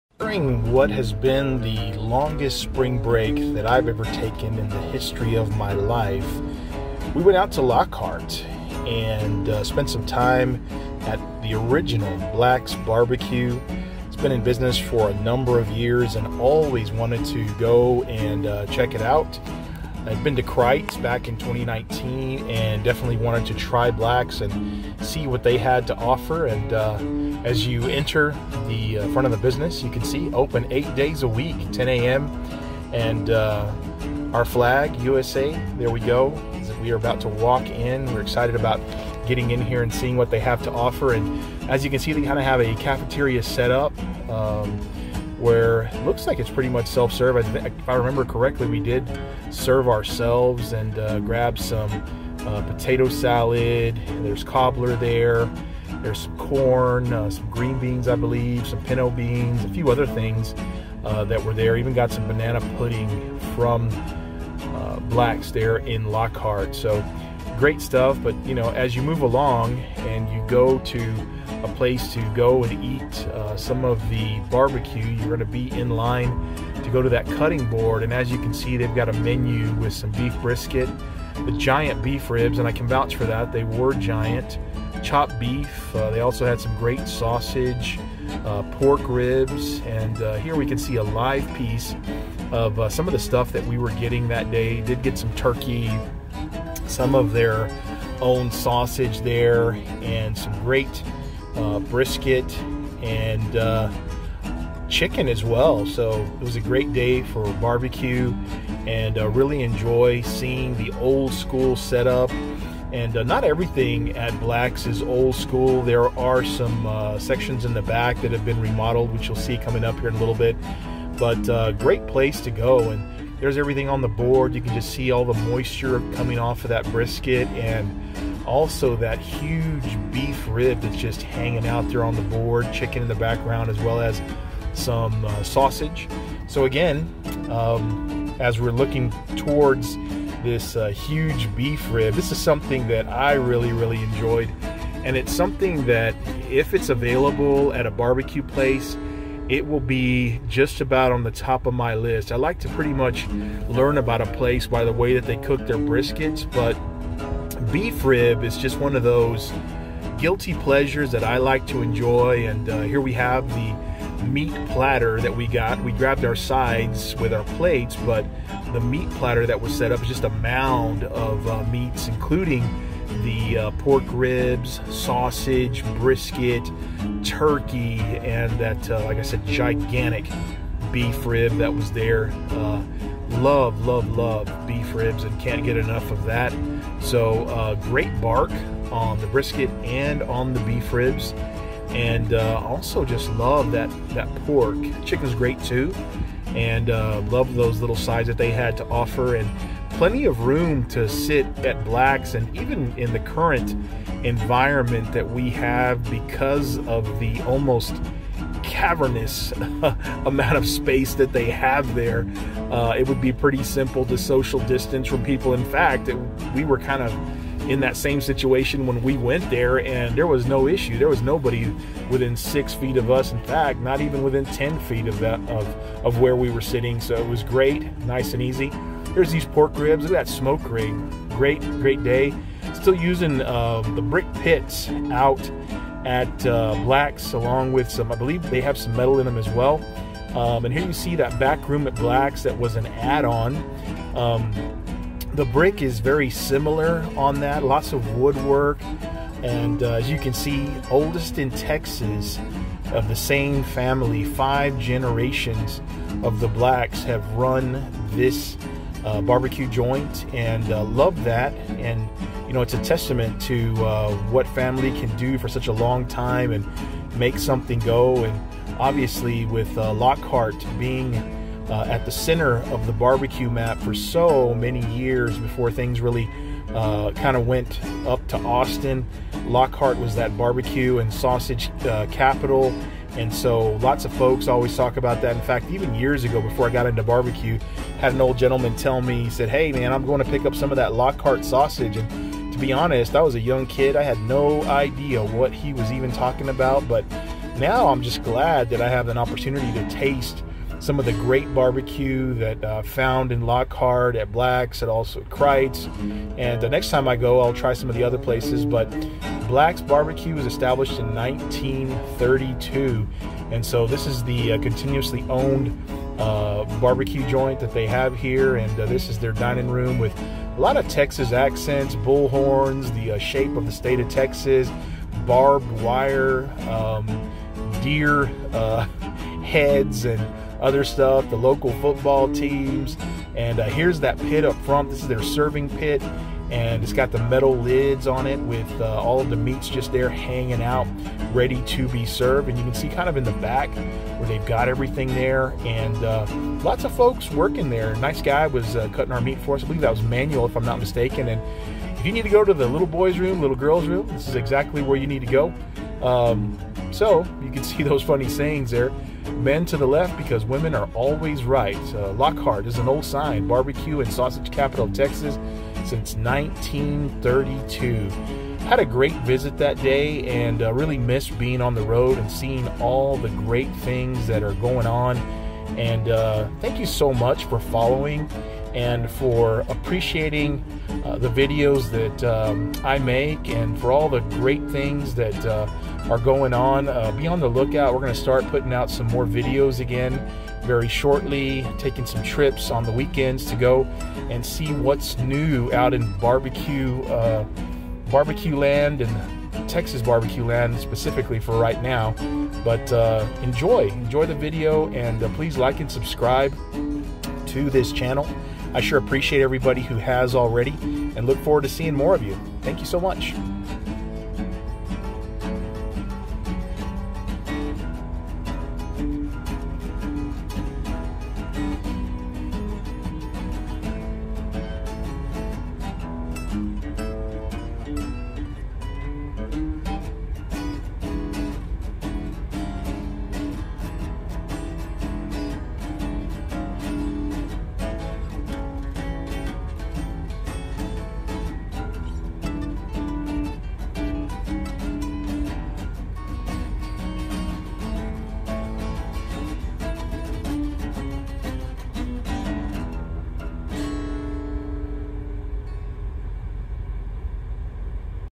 Black's BBQ - Lockhart, Texas - More Best BBQ in Texas
Another deep dive into BBQ Meat Porn in all its glory on my Spring break trip to Austin and Lockhart, Texas allowed some great opportunities to time travel through taste. I love being on the road with my family and seeing all the great things Central Texas has to offer! To taste BBQ in a place that has been around this long gives you an opportunity to time travel and sees what your ancestors tasted. So many people from all over the state have come to Lockhart for a taste of history and Black's is a place that many believe is one of the top BBQ joints in the history of BBQ and Texas.

Cook BBQ Like the pros in your own backyard

Pictures and videos: Mac Sedino